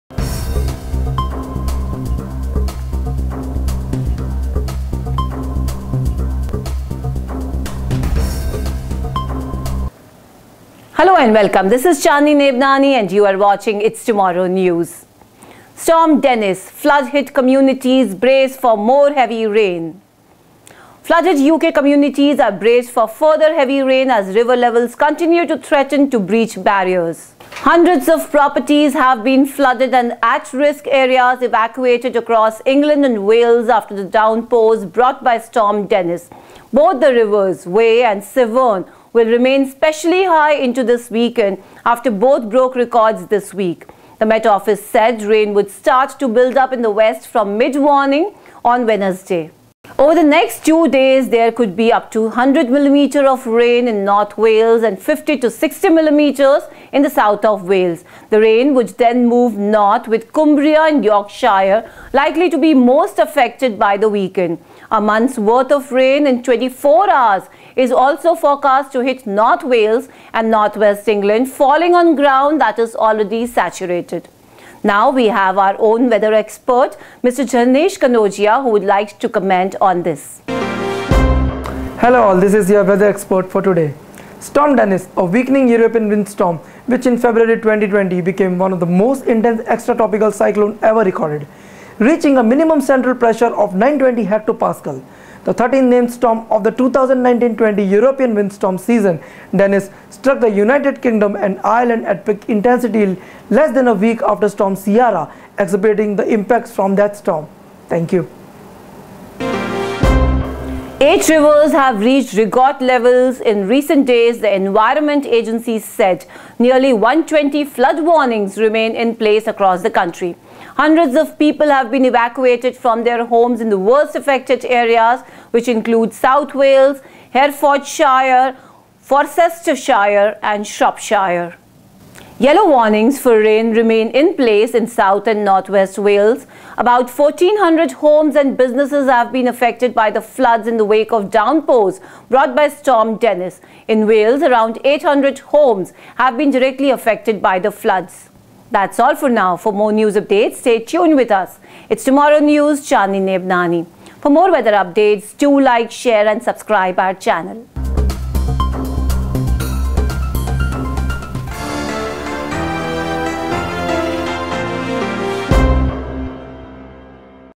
Storm Dennis flood hit communities brace for more heavy rain
Flood-hit UK communities are braced for further heavy rain as river levels continue to threaten to breach barriers. Hundreds of properties have been flooded and at-risk areas evacuated across England and Wales after the downpours brought by Storm Dennis. Both the rivers Wye and Severn will remain especially high into the weekend, after both broke records this week.